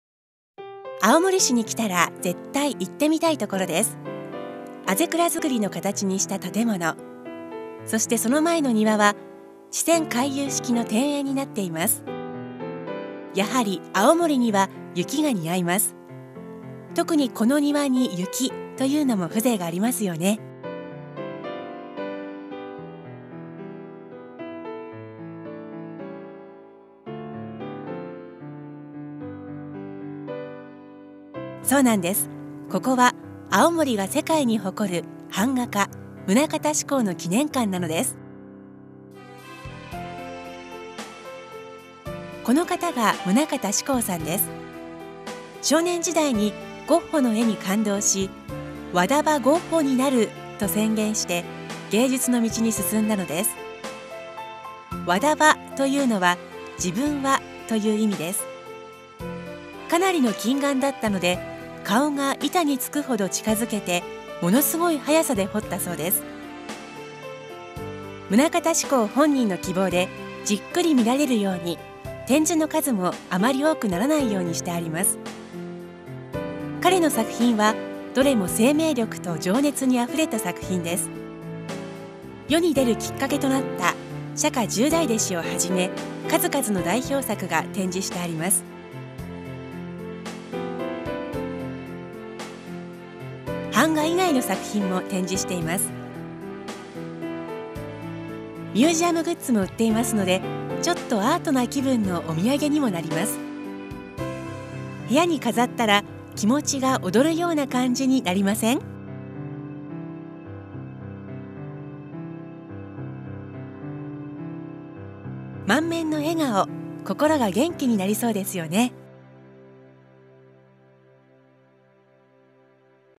棟方志功記念館 - 地域情報動画サイト 街ログ
青森が世界に誇る版画家・棟方志功の記念館。建物は校倉造り、庭は池泉回遊式の庭園になっている。世に出るきっかけとなった「釈迦十大弟子」をはじめとする数々の代表作のほか、版画以外の作品も展示している。